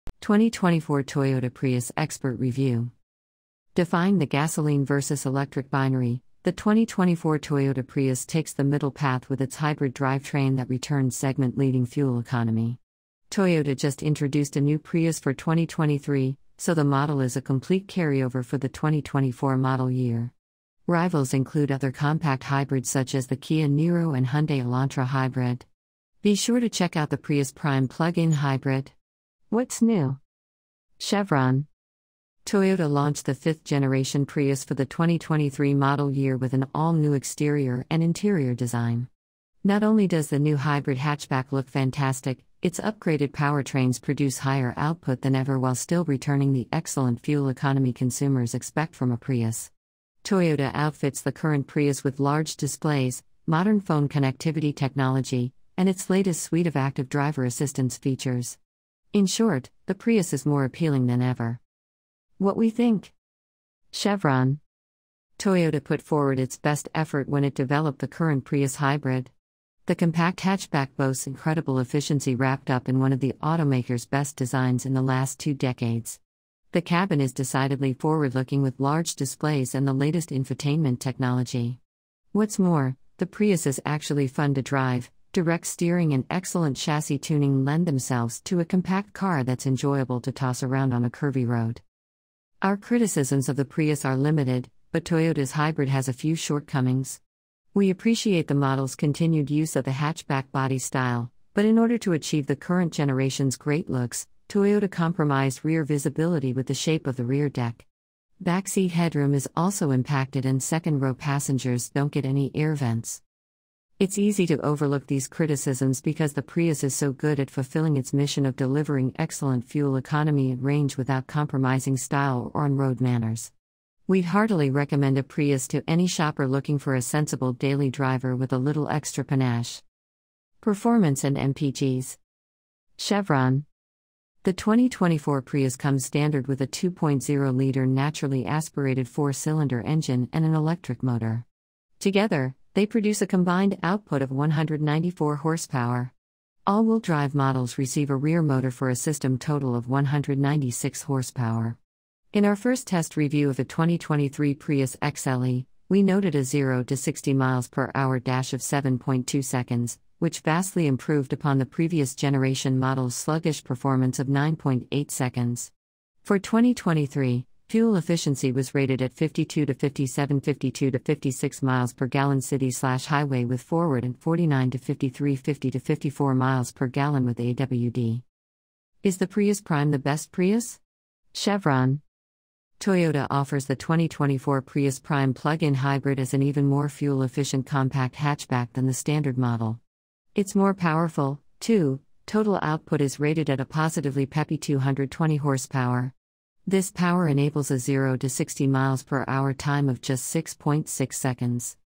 2024 Toyota Prius English Us
2024 Toyota Prius Expert Review ENGLISH US
Defying the gasoline versus electric binary, the 2024 Toyota Prius takes the middle path with its hybrid drivetrain that returns segment-leading fuel economy. Toyota just introduced a new Prius for 2023, so the model is a complete carryover for the 2024 model year. Rivals include other compact hybrids such as the Kia Niro and Hyundai Elantra Hybrid.

Be sure to check out the Prius Prime plug-in hybrid.
What's New
chevron
Toyota launched the fifth-generation Prius for the 2023 model year with an all-new exterior and interior design. Not only does the new hybrid hatchback look fantastic, its upgraded powertrains produce higher output than ever while still returning the excellent fuel economy consumers expect from a Prius. Toyota outfits the current Prius with large displays, modern phone connectivity technology, and its latest suite of active driver assistance features. In short, the Prius is more appealing than ever.

What We Think
chevron
Toyota put forward its best effort when it developed the current Prius hybrid. The compact hatchback boasts incredible efficiency wrapped up in one of the automaker's best designs in the last two decades. The cabin is decidedly forward-looking with large displays and the latest infotainment technology. What's more, the Prius is actually fun to drive; direct steering and excellent chassis tuning lend themselves to a compact car that's enjoyable to toss around on a curvy road.

Our criticisms of the Prius are limited, but Toyota's hybrid has a few shortcomings. We appreciate the model's continued use of the hatchback body style, but in order to achieve the current generation's great looks, Toyota compromised rear visibility with the shape of the rear deck. Backseat headroom is also impacted and second row passengers don't get any air vents.

It's easy to overlook these criticisms because the Prius is so good at fulfilling its mission of delivering excellent fuel economy and range without compromising style or on-road manners. We'd heartily recommend a Prius to any shopper looking for a sensible daily driver with a little extra panache.

Performance and MPGs
chevron
The 2024 Prius comes standard with a 2.0-liter naturally aspirated four-cylinder engine and an electric motor. Together, they produce a combined output of 194 hp. All-wheel-drive models receive a rear motor for a system total of 196 hp. In our First Test review of a 2023 Prius XLE, we noted a 0-60 mph dash of 7.2 seconds, which vastly improved upon the previous-generation model's sluggish performance of 9.8 seconds. For 2023, fuel efficiency was rated at 52-57/52-56 mpg city/highway with FWD and 49-53/50-54 mpg with AWD.

Is the Prius Prime the Best Prius?
chevron
Toyota offers the 2024 Prius Prime plug-in hybrid as an even more fuel-efficient compact hatchback than the standard model. It's more powerful, too; total output is rated at a positively peppy 220 hp. This power enables a 0-60 mph time of just 6.6 seconds. To supplement the Prius Prime's sportier character, Toyota equips model-specific wheels, clear taillight housing, and a red-accented interior. All-electric range is rated at 39 miles, which should suffice for most local trips or typical commuting. The Prime would be our pick of the Prius lineup, but it does come with a few disadvantages over the standard model, notably a higher price tag and lack of available AWD.

Safety Ratings and Features
chevron
Every 2024 Prius gets the latest version of Toyota Safety Sense 3.0, the brand's suite of active driver assistance features. Standard features include pre-collision avoidance with pedestrian detection, lane departure avoidance, adaptive cruise control, lane-following assistance, road sign assist, and automatic high-beams. The suite bundles in Toyota's Proactive Driving Assist as well, which utilizes the exterior cameras to determine when braking and steering assistance are necessary. Toyota also offers a 360-degree camera, front and rear parking sensors with automatic braking, blind-spot monitoring with rear cross-traffic alert, and hands-free parking assistance.

  Legroom (front/rear) Cargo Space (seats up)
2024 Toyota Prius 43.2/34.8 inches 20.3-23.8 cubic feet
2023 Kia Niro 41.5/39.8 inches 22.8 cubic feet
2023 Hyundai Kona Electric 41.5/33.4 inches 19.2 cubic feet
Technology
chevron
Toyota equips the 2024 Prius with a standard 8.0-inch infotainment unit with wireless Apple CarPlay and Android Auto. It offers a 12.3-inch infotainment display on higher trims. A 7.0-inch fully digital instrument cluster is standard across the entire range, which helps the cabin's styling conform with the all-electric bZ4X SUV. Other available tech includes satellite radio, a Wi-Fi hot spot, and an integrated version of Apple Music and Amazon Music. A six-speaker sound system is standard and an eight-speaker arrangement from JBL comes equipped on the top-level Limited model.